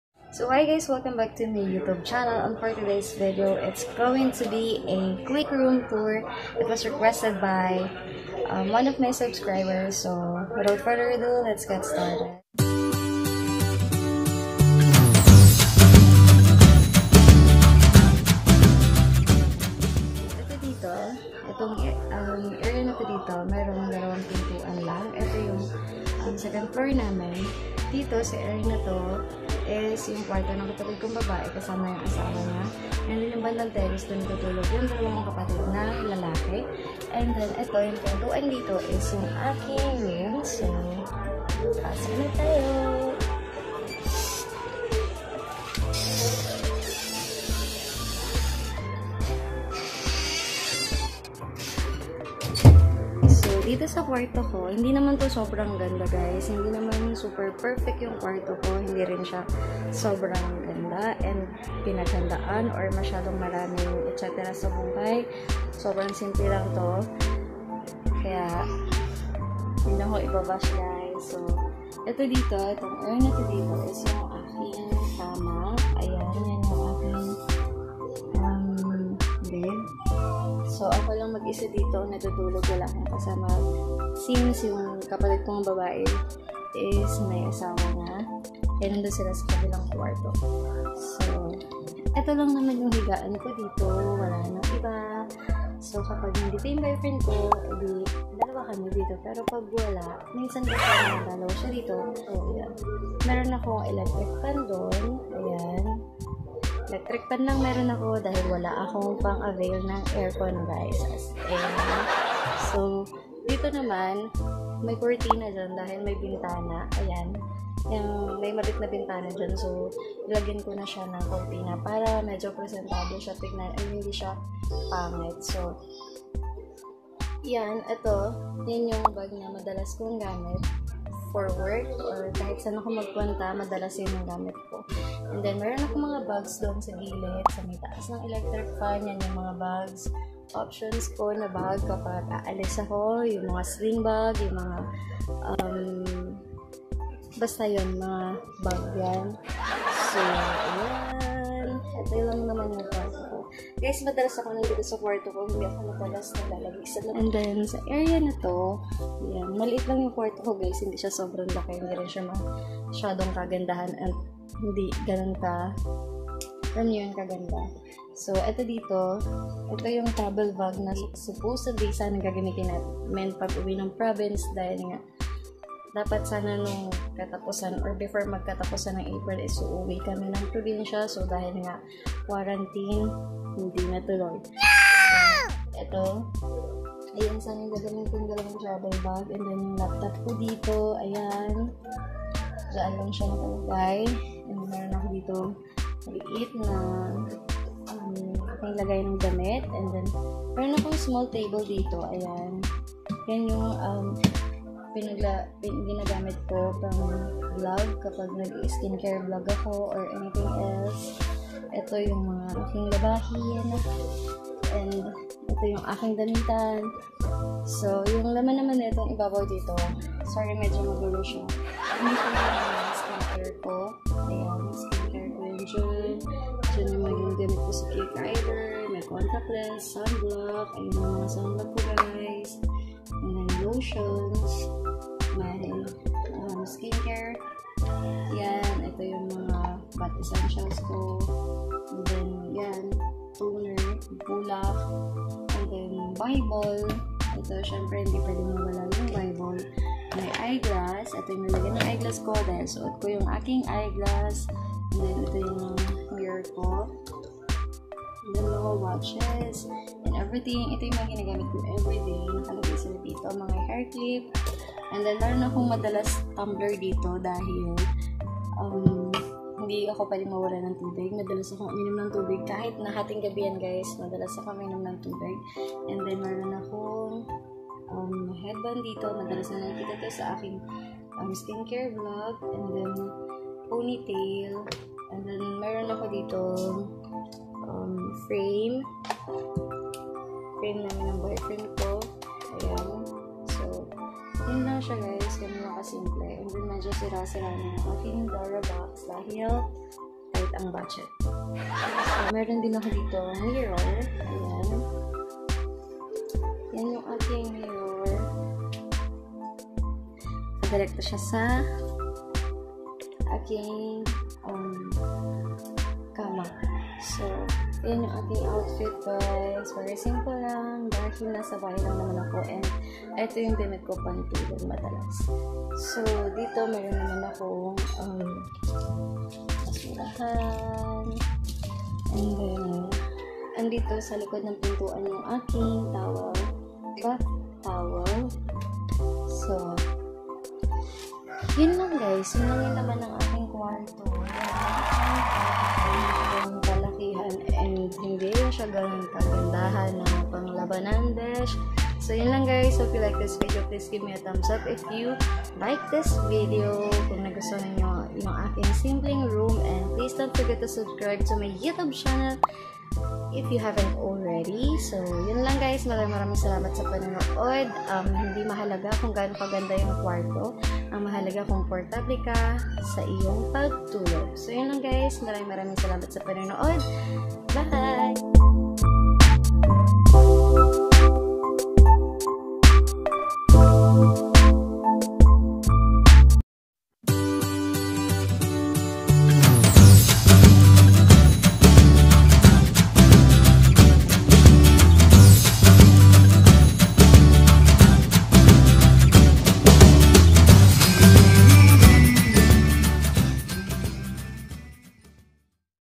MY ROOM TOUR//my simple room//Crischelle's vlog
Hi there everyone my room wasn't that perfect!!! Hope you all appreciate my simple room enjoy watching!!! Stay safe every one stay at home be good!!! Spread the love!